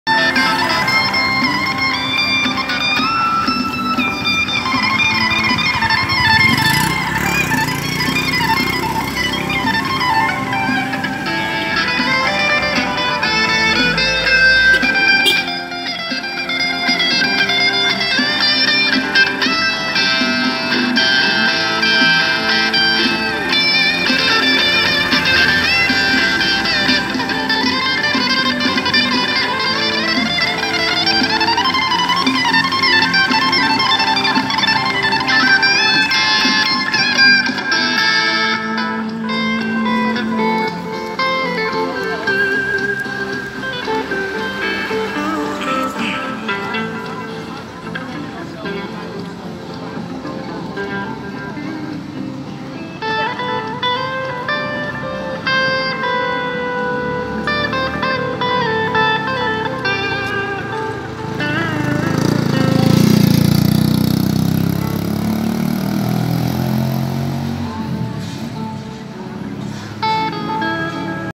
Artista de rua em Oiapoque
Durante um passeio pela manhã de sábado no centro comercial de Oiapoque encontrei este rapaz tocando na esquina do Banco do Brasil, aproveitei para fazer este registro. Na ocasião ele me deu um cartão da página dele que irei disponibilizar mais tarde.